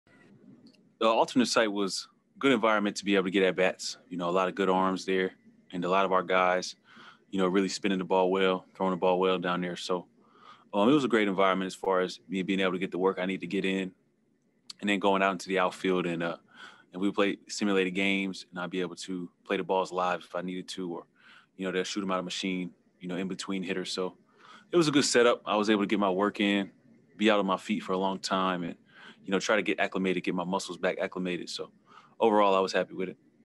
Kyle Lewis on Alternate Site 2021-04-22
When Mariner players are injured, they're sent to the alternate site in Tacoma to rehab.  While they don't see any game-action, they see plenty of live pitching and play defense.  Such was the case for Seattle's Kyle Lewis, who spent the first three weeks of the season rehabbing in Tacoma.  He says it worked out well for him and he's happy to be back.